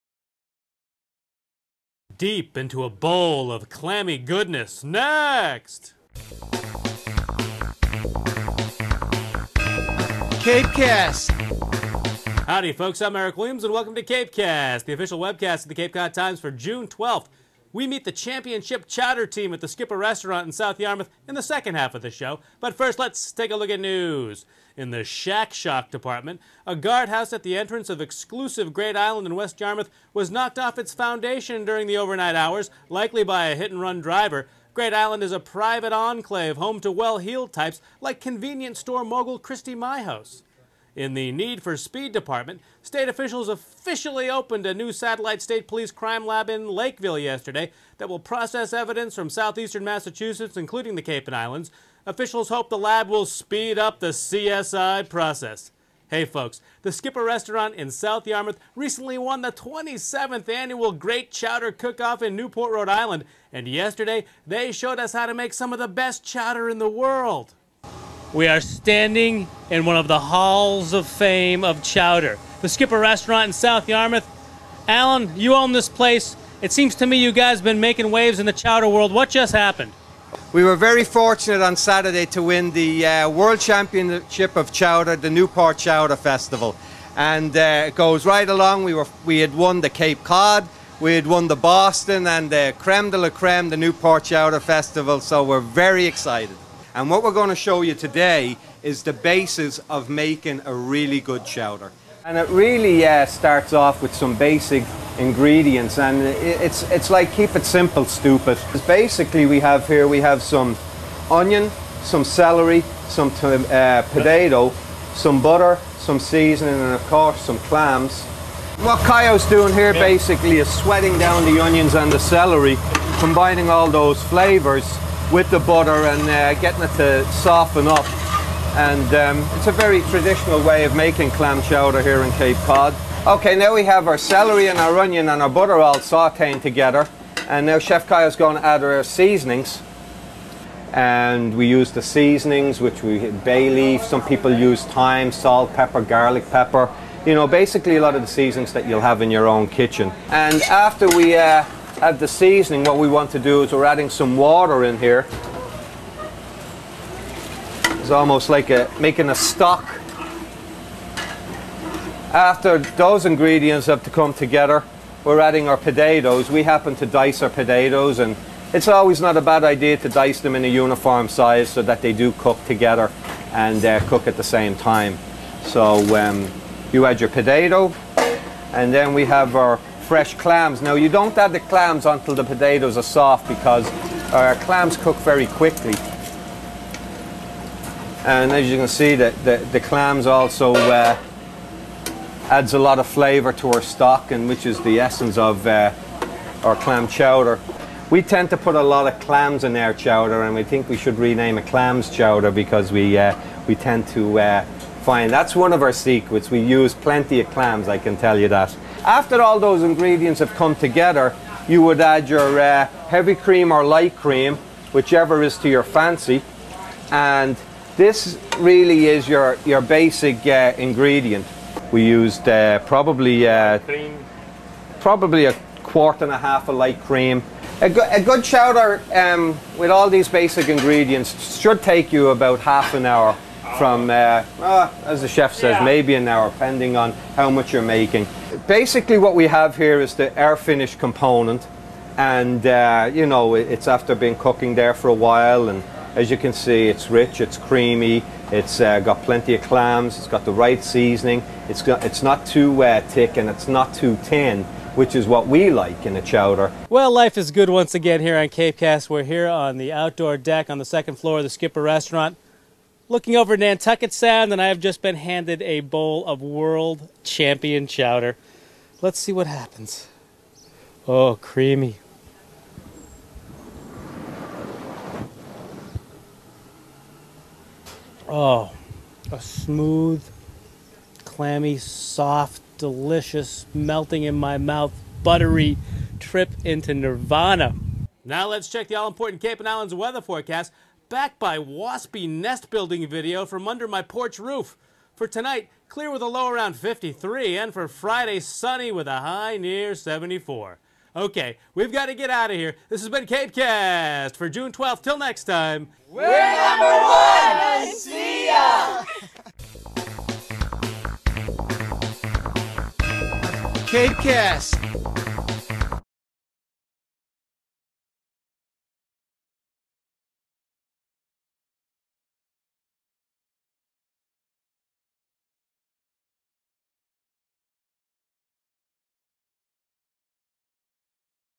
Super bowl of clam chowder!
On today's CapeCast: We grab our spoons and head deep into a bowl of chowder at the Skipper Restaurant in South Yarmouth, winners of the prestigious, delicious Great Chowder Cook-Off in Newport.